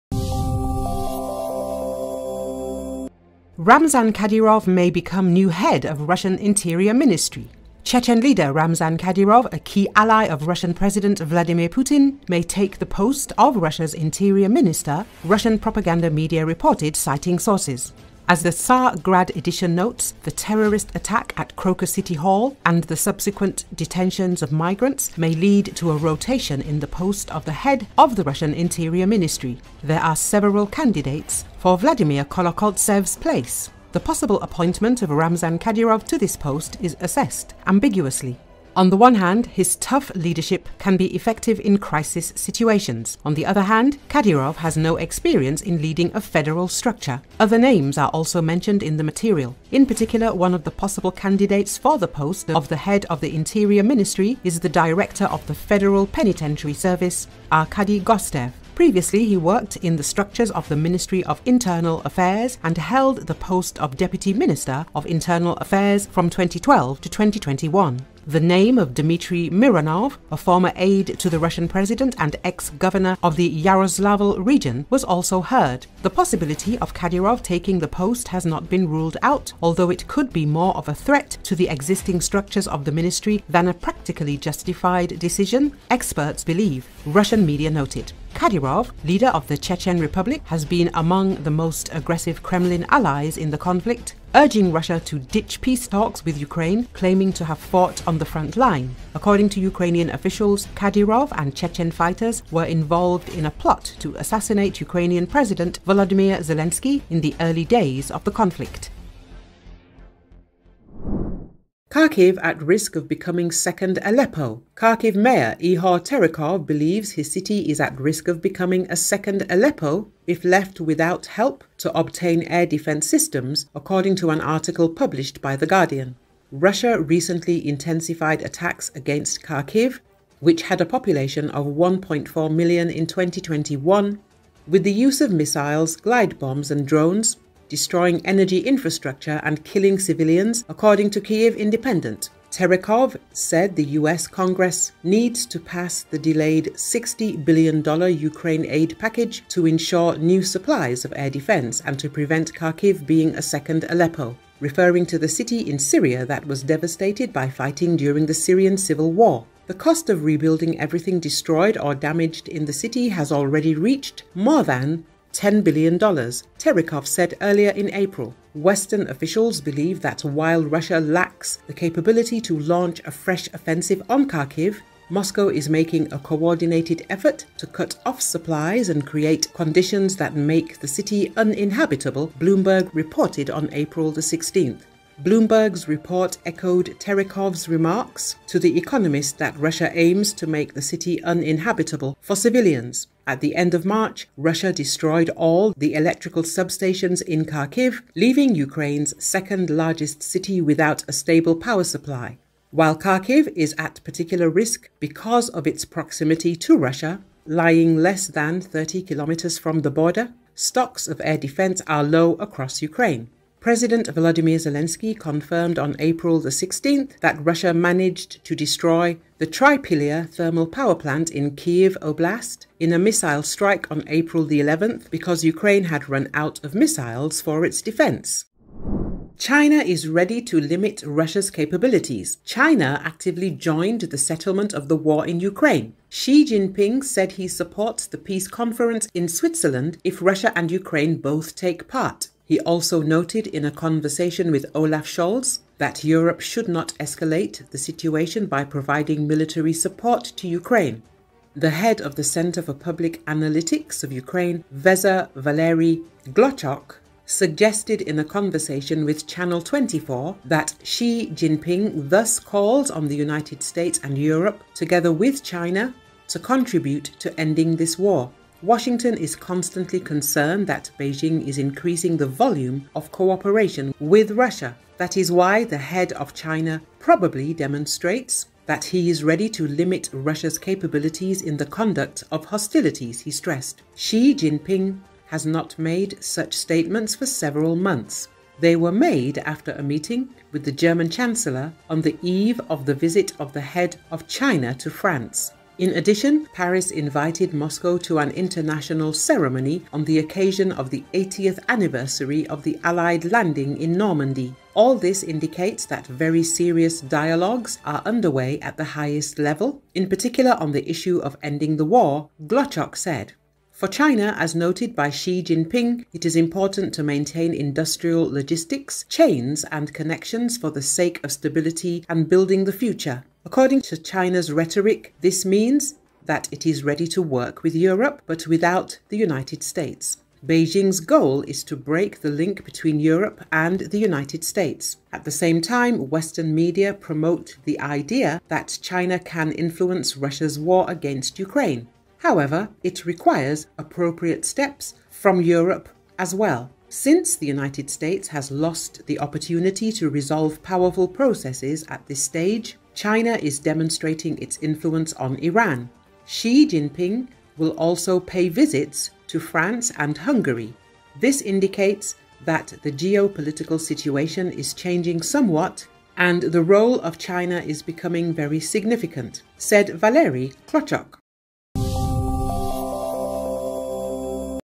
Ramzan Kadyrov may become next Russian interior minister
Chechen leader Ramzan Kadyrov, a key ally of Russian President Vladimir Putin, may take the post of Russia's interior minister, Russian propaganda media reported, citing sources.
As the Tsargrad edition notes, the terrorist attack at Crocus City Hall and the subsequent detentions of migrants may lead to a rotation in the post of the head of the Russian Interior Ministry. There are several candidates for Vladimir Kolokoltsev's place.
The possible appointment of Ramzan Kadyrov to this post is assessed ambiguously. On the one hand, his tough leadership can be effective in crisis situations. On the other hand, Kadyrov has no experience in leading a federal structure.
Other names are also mentioned in the material. In particular, one of the possible candidates for the post of head of the Interior Ministry is the director of the Federal Penitentiary Service Arkady Gostev. Previously he worked in the structures of the Ministry of Internal Affairs and held the post of Deputy Minister of Internal Affairs from 2012 to 2021.
The name of Dmitry Mironov, a former aide to the Russian president and ex-governor of the Yaroslavl region, was also heard.
"The possibility of Kadyrov taking the post has not been ruled out, although it could be more of a threat to the existing structures of the ministry than a practically justified decision, experts believe," Russian media noted.
Kadyrov, leader of the Chechen Republic, has been among the most aggressive Kremlin allies in the conflict, urging Russia to ditch peace talks with Ukraine, claiming to have fought on the front line. According to Ukrainian officials, Kadyrov and Chechen fighters were involved in a plot to assassinate Ukrainian President Volodymyr Zelensky in the early days of the conflict.